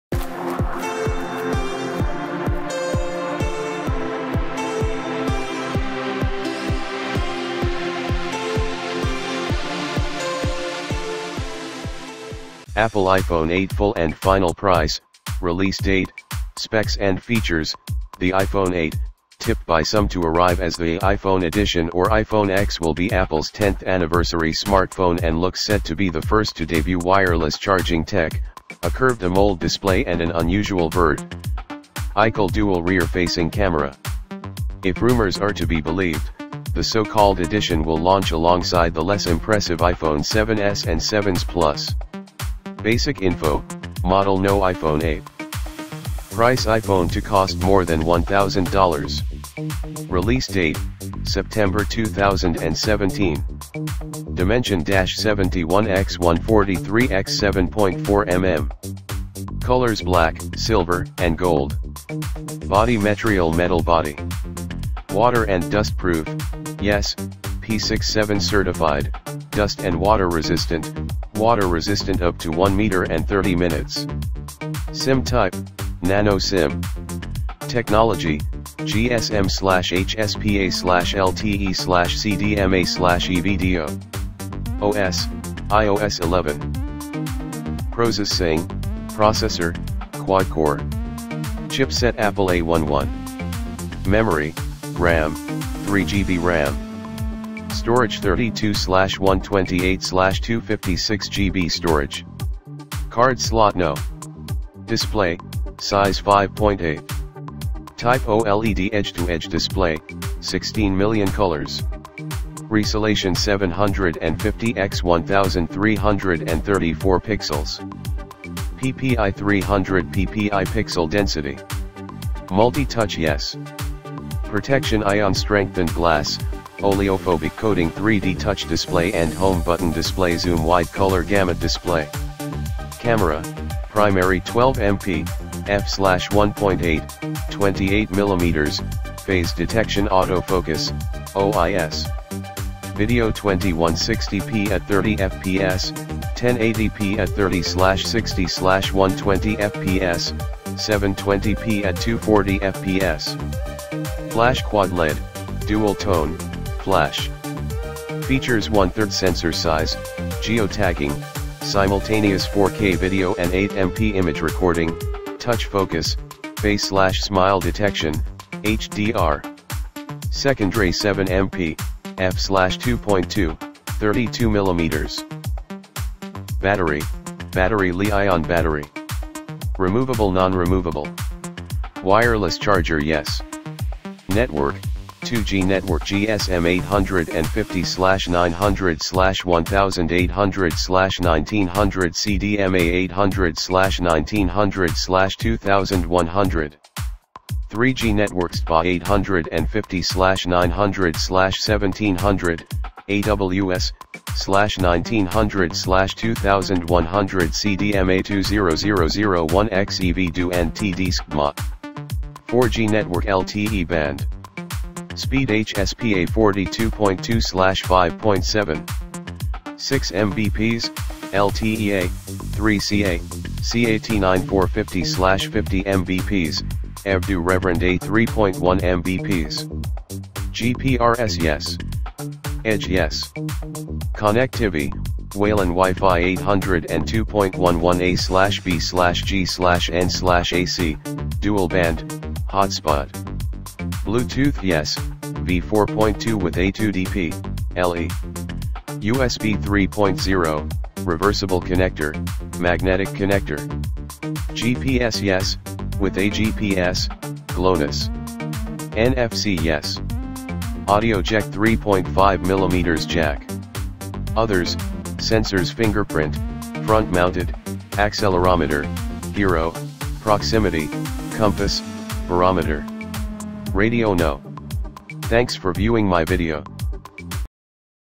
Apple iPhone 8 Full & Final । Specs & Features
The iPhone 8, tipped by some to arrive as the 'iPhone Edition' or 'iPhone X' will be Apple's
10th-anniversary smartphone and looks set to be the first to debut wireless charging tech,
a curved AMOLED display and an unusual vertical dual rear-facing camera.
If rumours are to be believed, the so-called 'Edition' will launch alongside the less-impressive iPhone 7S and 7S Plus.

BASIC INFO :
Model No -iPhone 8.
Price- iPhone to cost more than $1,000.
Release Date : September 2017.
Dimension -71x143x7.4mm.
Colors-  Black, Silver and Gold.
Body Metrial -Metal body.
Water & Dust Proof - Yes, P67 certified - dust and water resistant - Water resistant up to 1 meter and 30 minutes.
Technology - GSM / HSPA / LTE / CDMA / EVDO.
OS - iOS 11.

PROSESSING :
Processor - Quad core.
Chipset- Apple A11.

MEMORY :
RAM - 3GB Ram.
Storage -32/128/256GB storage.
Card Slot-No.

DISPLAY :
Size- 5.8".
Type-  OLED edge-to-edge display, 16 Million colours.
Resolation- 750 x 1334 pixels.
PPI- 300 ppi pixel density.
Multi touch- Yes.
Protection- Ion-strengthened glass,
oleophobic coating
3D Touch display
& home button
Display Zoom
Wide color gamut display.

CAMERA :
Primary- 12 MP, f/1.8, 28mm, phase detection autofocus, OIS.
Video- 2160p@30fps, 1080p@30/60/120fps, 720p@240fps.
Flash- Quad-LED (dual tone) flash.
Features- 1/3 sensor size, geo-tagging, simultaneous 4K video and 8MP image recording, touch focus, face/smile detection, HDR.
Secondray- 7 MP, f/2.2, 32mm.

BATTERY :
Battery- Li-ion battery.
Capacity -
Removable- Non-removable.
Wireless Charger- Yes.

NETWORK :
2G Network- GSM 850 / 900 / 1800 / 1900
CDMA 800 / 1900 / 2100.
3G Network- HSDPA 850 / 900 / 1700(AWS) / 1900 / 2100
CDMA2000 1xEV-DO & TD-SCDMA.
4G Network- LTE band.
Speed- HSPA 42.2/5.76 Mbps,
LTE-A (3CA) Cat9 450/50 Mbps,
EV-DO Rev.A 3.1 Mbps.
GPRS- Yes.
EDGE- Yes.

CONECTIVY :
WALAN- Wi-Fi 802.11 a/b/g/n/ac, dual-band, hotspot.
BlueTooth- Yes, v4.2 with A2DP, LE.
USB- 3.0, reversible connector; magnetic connector.
GPS- Yes, with A-GPS, GLONASS.
NFC- Yes.
Audio jeck- 3.5mm Jack.

OTHERS :
Sensors- Fingerprint (front-mounted), accelerometer, gyro, proximity, compass, barometer.
Radio- No.